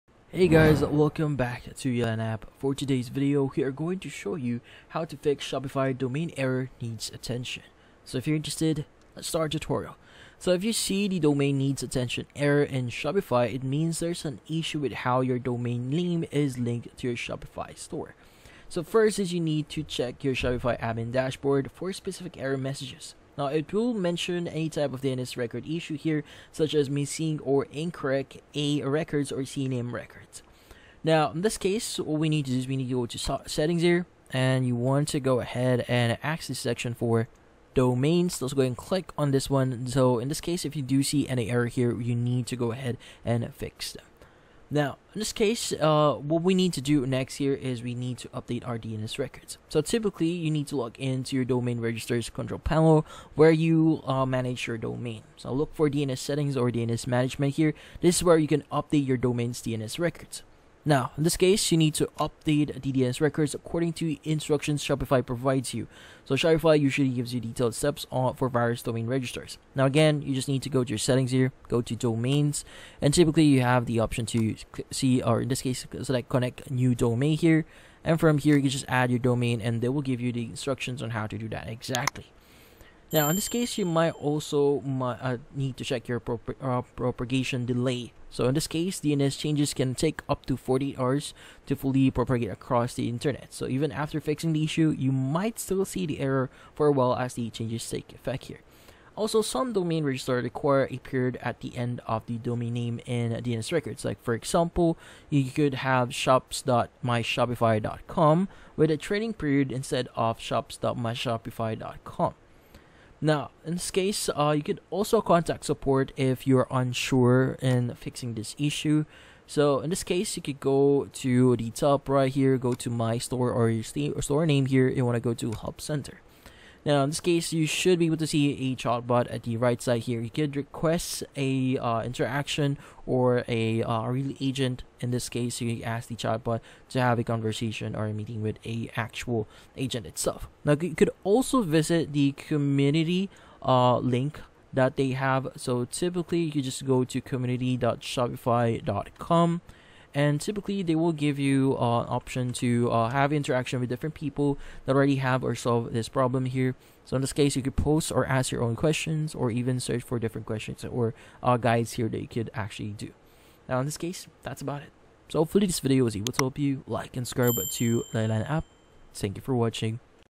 How To Fix Shopify Domain Error Needs Attention (2025)
If you want to be able to fix shopify domain error needs attention, then this video will be perfect for you!